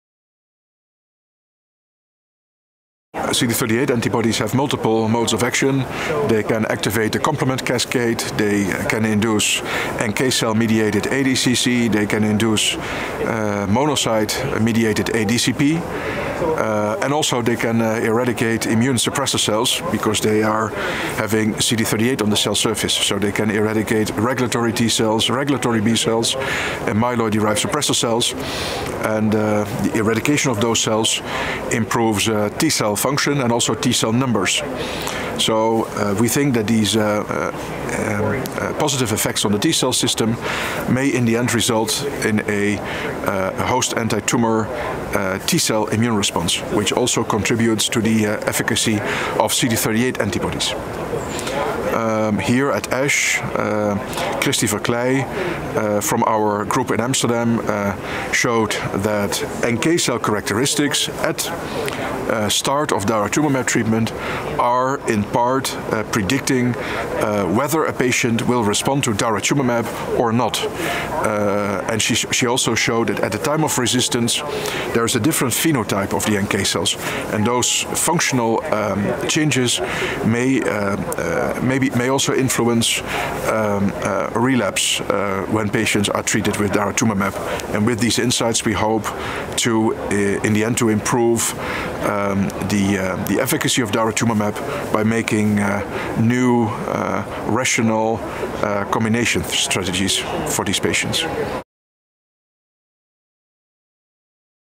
Mechanisms of action of CD38 antibodies in myeloma
Niels van de Donk, MD, PhD, VU University Medical Center, Amsterdam, Netherlands, discusses the mechanisms of action of CD38 antibodies in multiple myeloma. CD38 antibodies are thought to exert their anti-cancer activity through numerous modes of action, including the induction of NK-cell mediated antibody-dependent cell-mediated cytotoxicity (ADCC) and antibody-dependent cellular phagocytosis (ADCP) by monocytes, as well as immune suppressor cell eradication. Prof. van de Donk comments on recent findings suggesting that NK-cell characteristics may be predictive of responses to CD83-targeted agents and how these insights may be able to improve treatment efficacy. This interview took place at the 63rd ASH Annual Meeting and Exposition congress in Atlanta, GA.